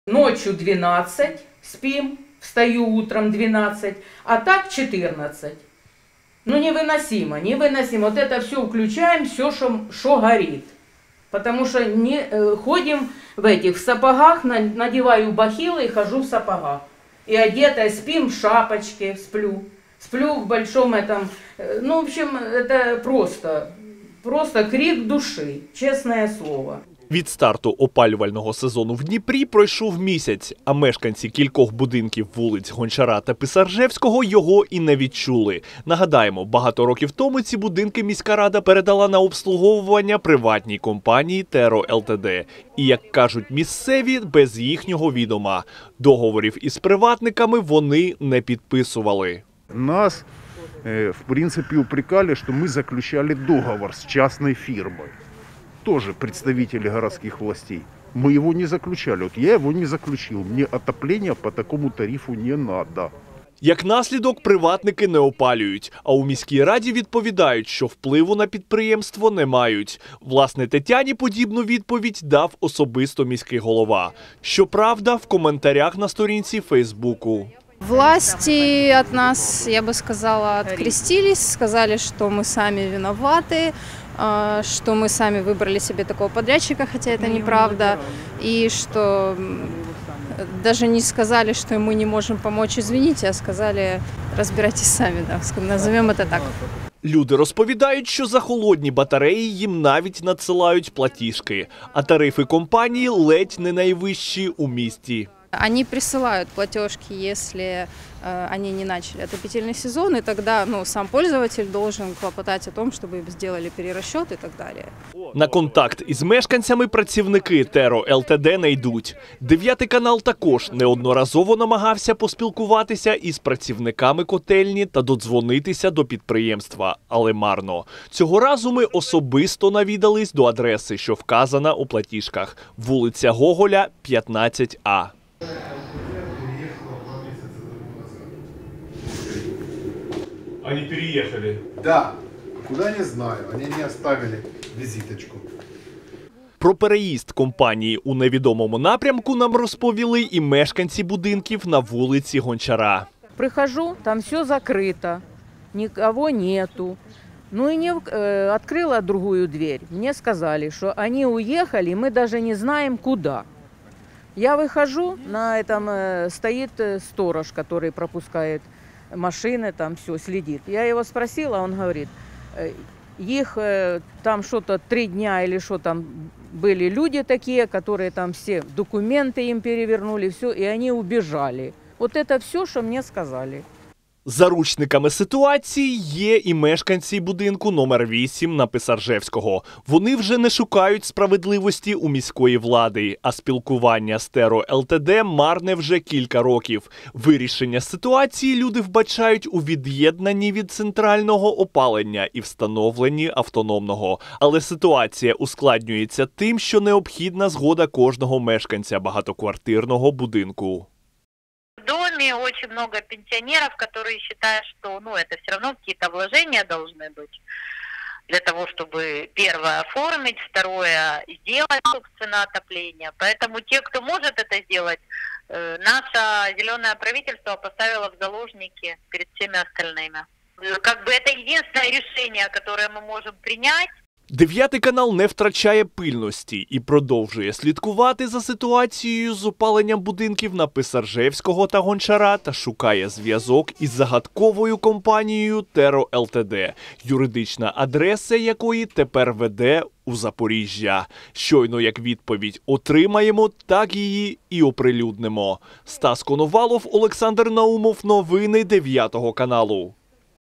"Терро інкогніто": мешканці вулиць Гончара і Писаржевського шукають сліди постачальників тепла
Опалювальна епопея мешканців вулиць Писаржевського та Гончара продовжується. Їм доводиться щороку буквально виборювати те, щоб мати у своїх оселях теплі батареї. Будинки обслуговує приватна компанія, як наслідок — батареї холодні, а їхні представники із споживачами на контакт не йдуть